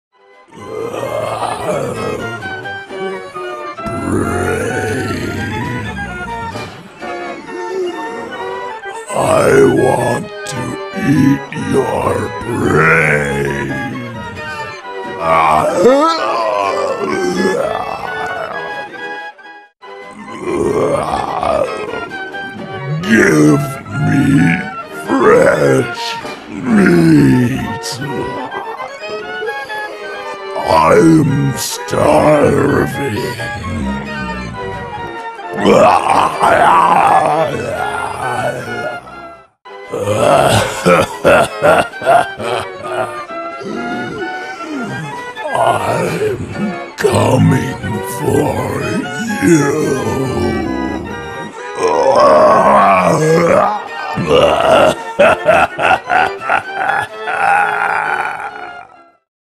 Zombie Clown Animated
6 foot tall Zombie clown character with metal frame, slush-PVC head and hands, blow molded chest ribcage, synthetic hair, fabric costume, motor that slowly raises & lowers one arm while torso slowly rocks from side-to-side & head turns from side-to-side; motion sensor activated green LED light-up eye with painted PVC plastic cap, with creepy sounds; Fabric shoes. Right hand is holding a balloon or whatever prop that you may have flowers, cotton candy, ect